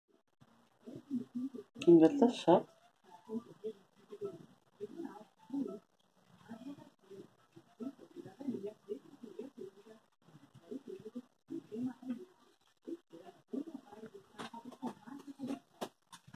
Dicentria sp. caterpillars
Lagartas de mariposa do gênero Dicentria. Elas estão fazendo ecdise.

Notodontidae

Caterpillars of Dicentria moth. They are doing ecdise